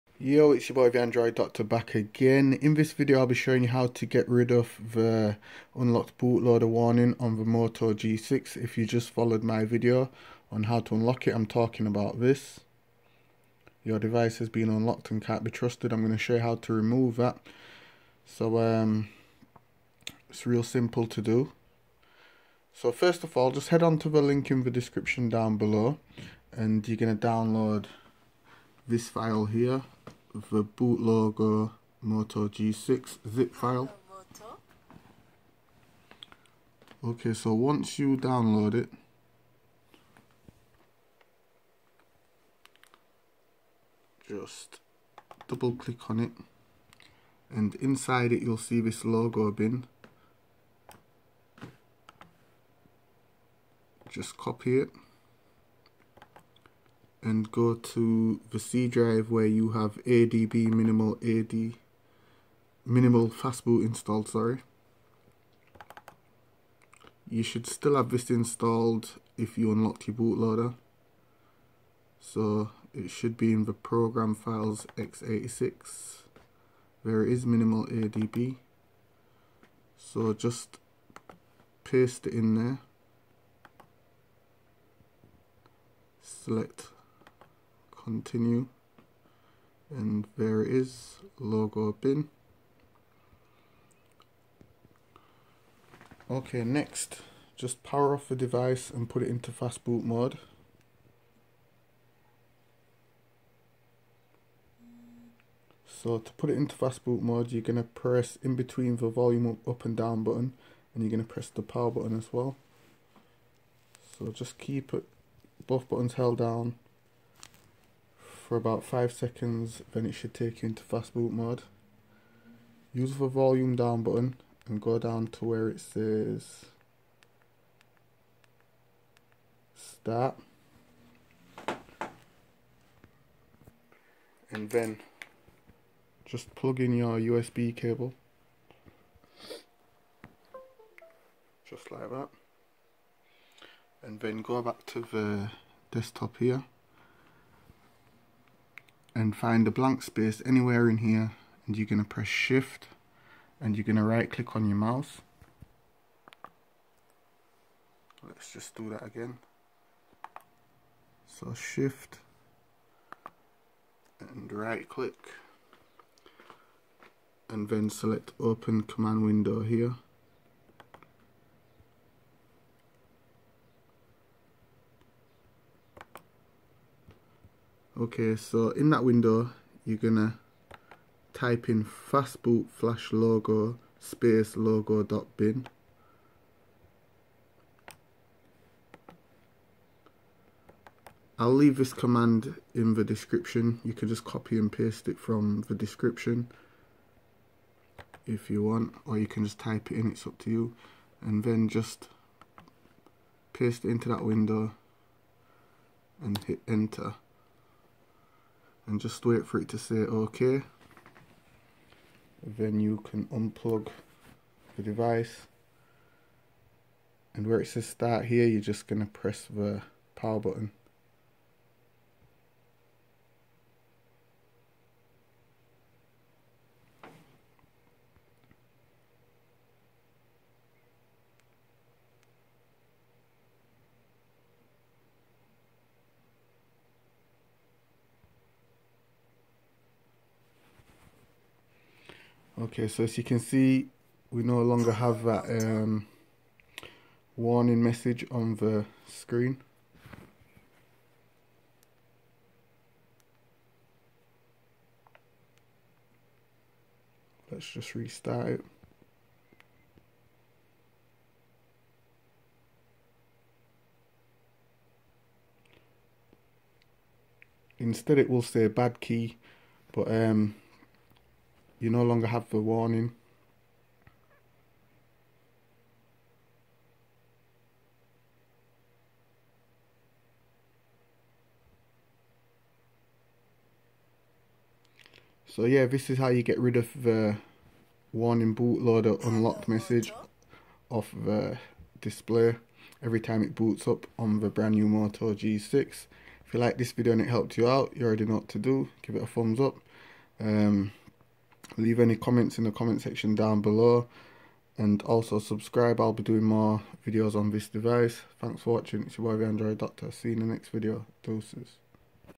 Moto G6 Remove Bootloader Unlocked Warning
Warning u do this at your own risk i will not be held responsible if anything bad happens to your device!!!

Type this command into the adb fastboot  black window
fastboot flash logo logo.bin

IF U WANT Samsung Galaxy S9+ Edge Screen Panel On Any Android NO ROOT NEEDED Watch